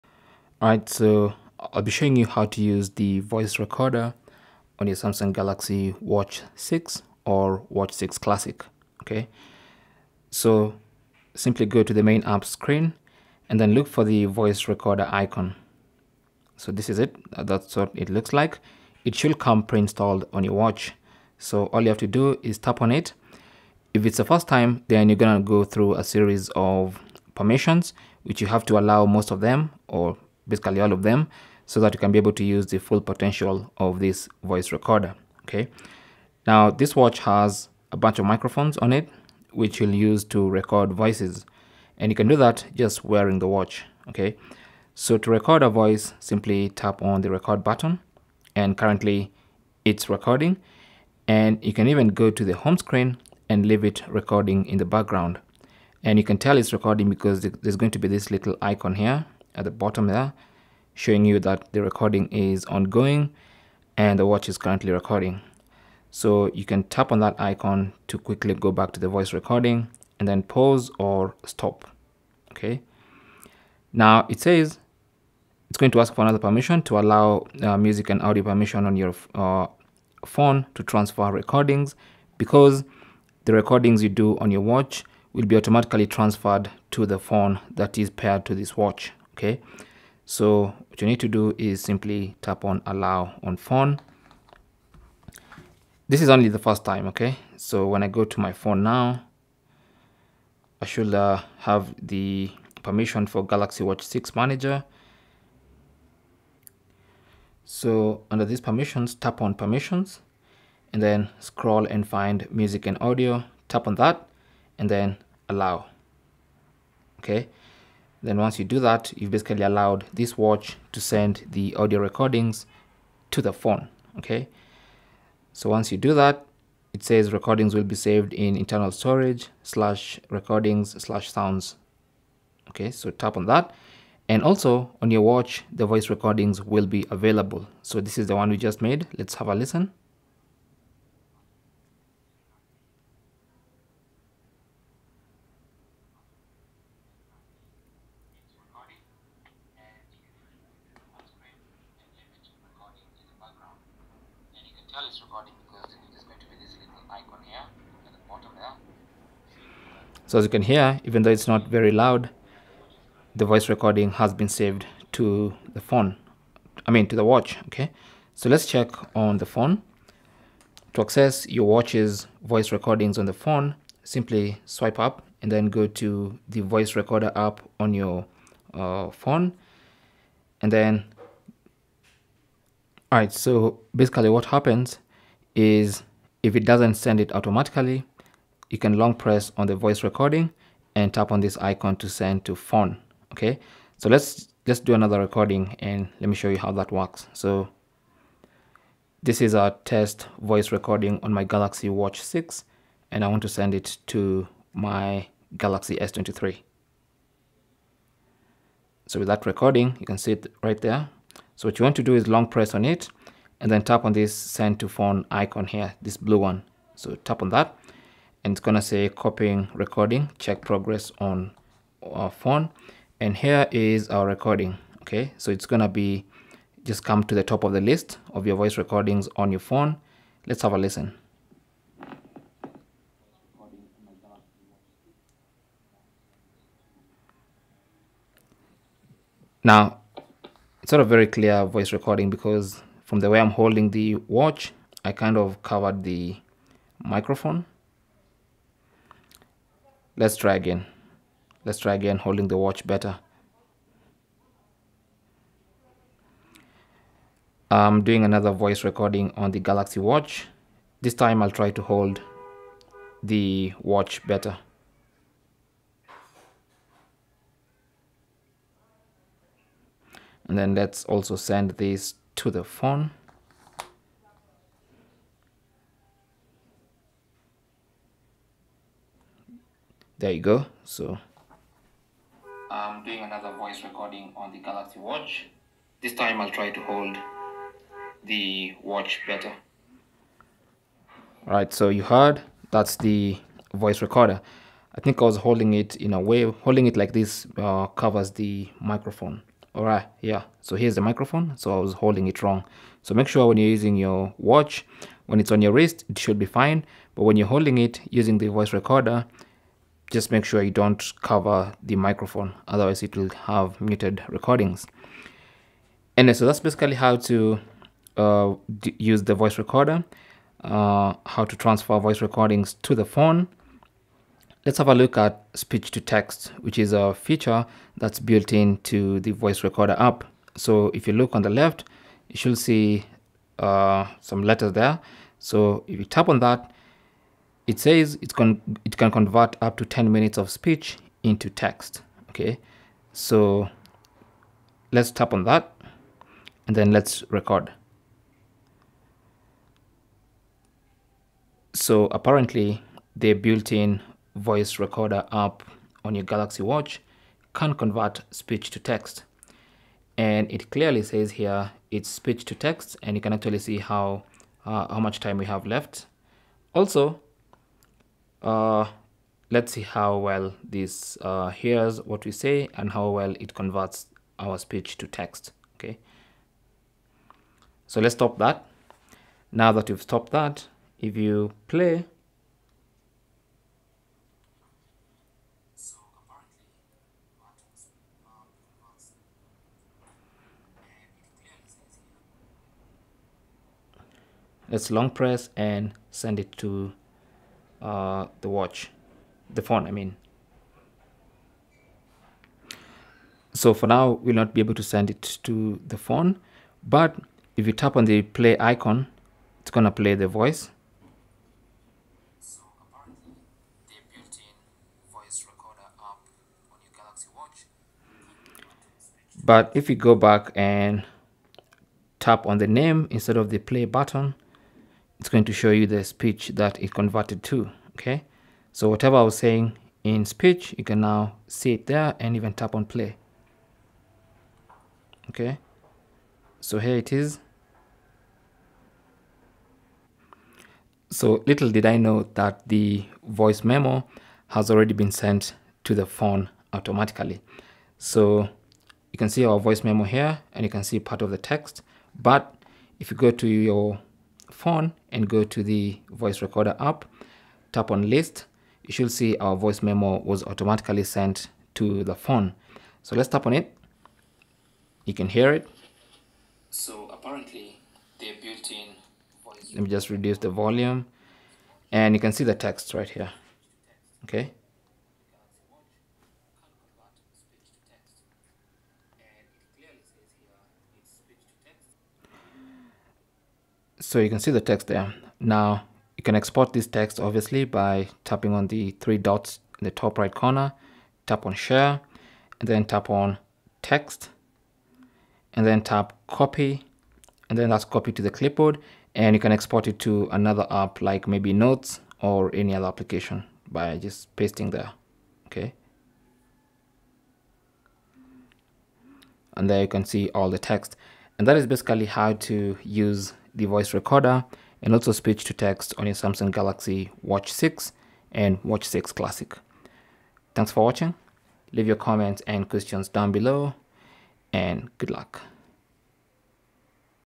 How To Use Voice Recorder On Samsung Galaxy Watch 6 /6 Classic (Record, Transfer Speech To Text)
Learn how to use a voice recorder on the Samsung Galaxy Watch 6 and 6 Classic.

The built-in voice recorder on the Samsung Galaxy Watch 6 allows you to capture audio recordings right from your wrist. This tutorial video will walk you through the steps to use the voice recorder on your Samsung Galaxy Watch 6 or 6 Classic, as well as how to transfer and convert those recordings into text.

How To Use Voice Recording On Samsung Watch 6
Find the Voice recorder app
Tap on the red icon to begin recording

Timestamps
0:00 - Introduction
0:10 - How to use the voice recorder on Samsung Galaxy Watch 6
5:24 - How to use the speech-to-text feature on the Samsung Galaxy Watch 6
8:30 - Conclusion

Samsung Galaxy Z Flip 5:

Samsung Galaxy Watch 6/ 6 Classic: (Customize Your Order):

Galaxy S23 Ultra DUAL SIM 256GB 8GB Phantom Black:

Cell Phone Tripod Adapter